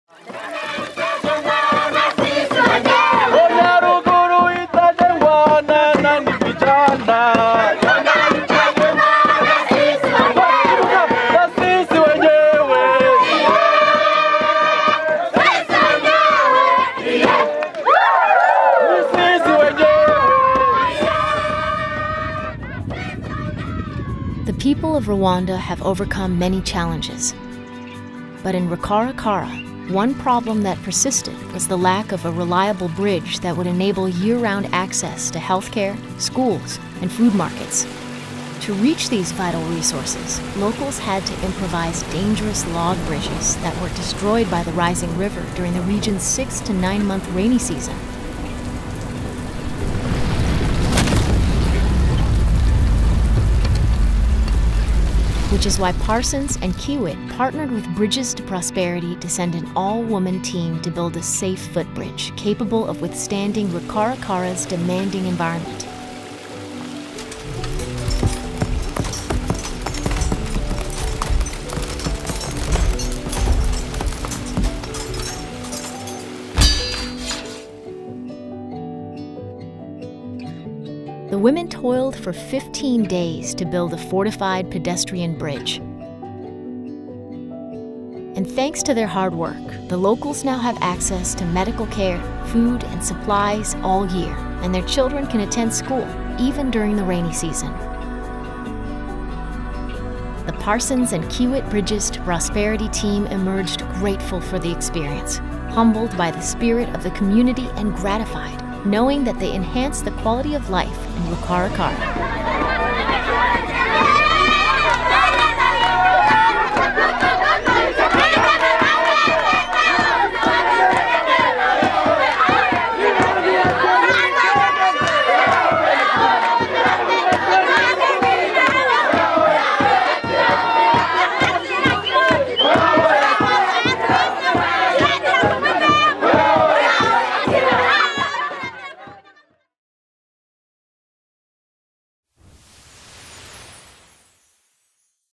Transforming Lives in Rwanda (2019)
Parsons and Kiewit team with Bridges to Prosperity to build a bridge connecting villagers to vital services.